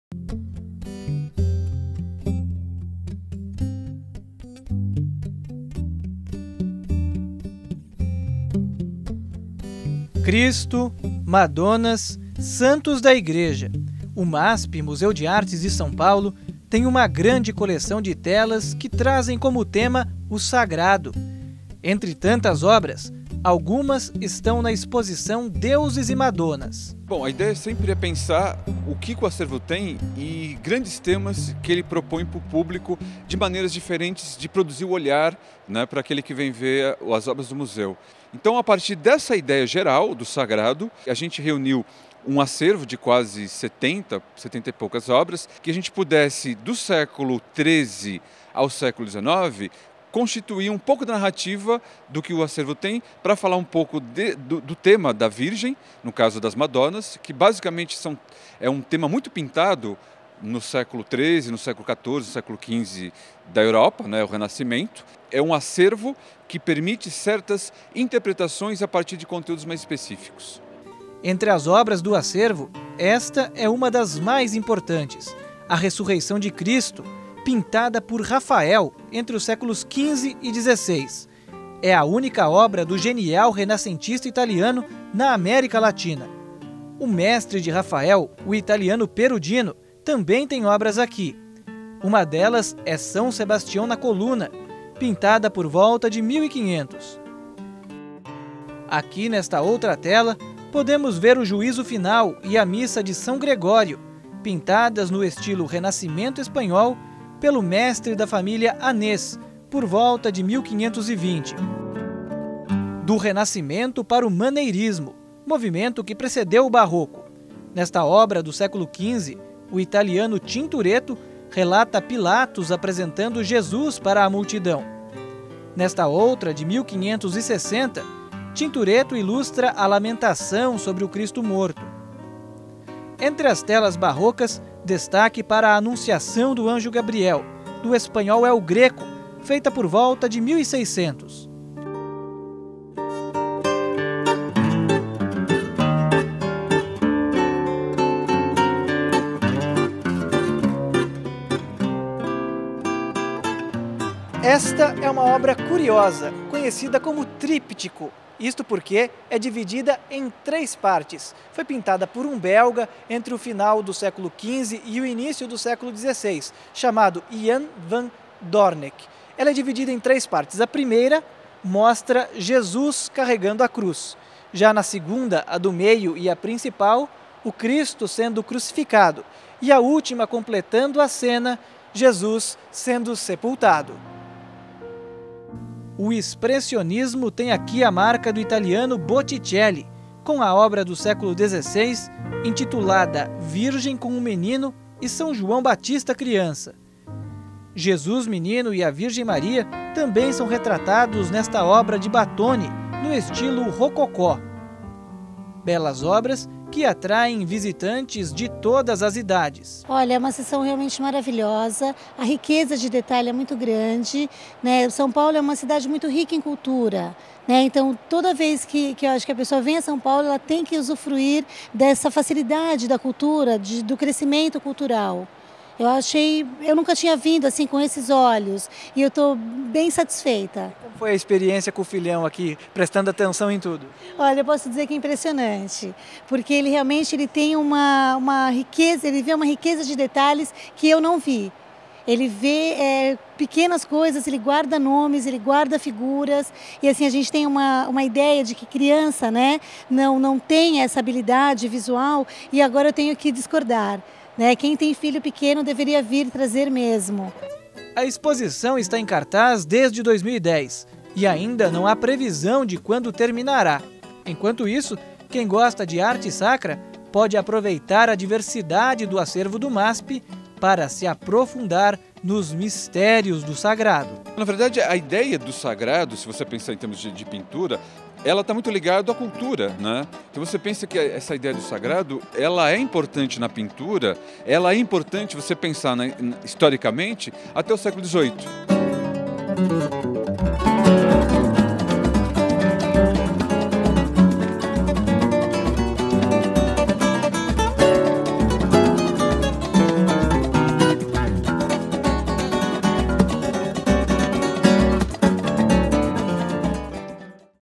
Reportagem | Exposição Deuses e Madonas
PROGRAMA FAMÍLIA EM FOCO
Deuses e Madonas
Reporter: Leandro Barbosa
Deuses e Madonas — A Arte do Sagrado é o título da mostra que reúne quarenta obras, de pintores famosos como El Greco,  Rafael, Botticelli e outros. A mostra é uma viagem pelo universo do sagrado na cultura ocidental através de obras dos século 14 ao 19.
Visite você também exposição com sua família, será uma oportunidade de aprender junto com seus filhos sobre a história da arte e da religião.